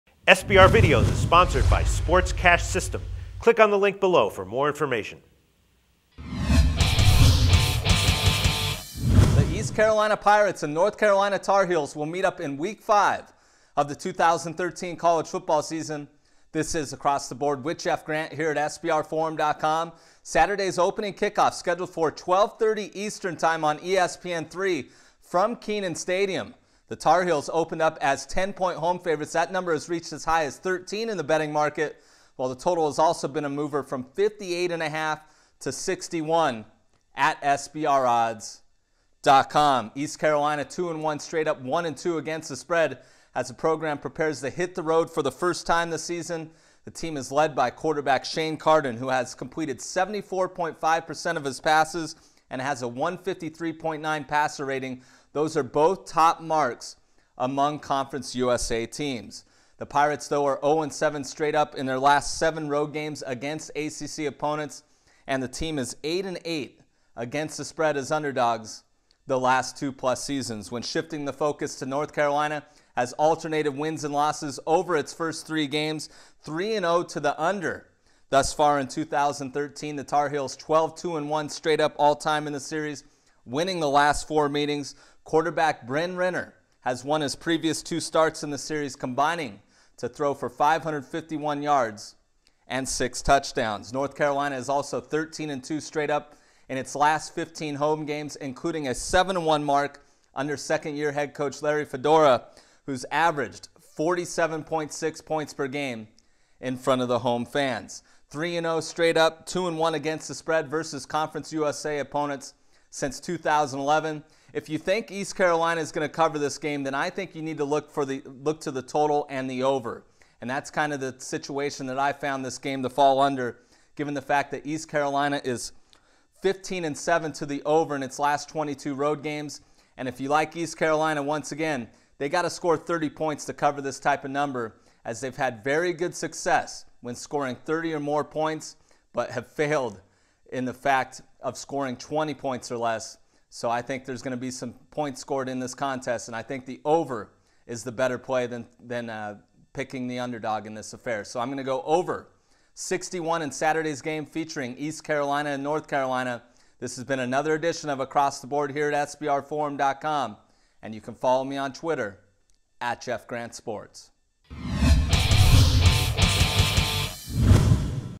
College Football Picks: East Carolina Pirates vs. North Carolina Tar Heels
The North Carolina Tar Heels will have an advantage when they play the East Carolina Pirates. But how much will the College football odds reflect that and will it be profitable for our College Football picks?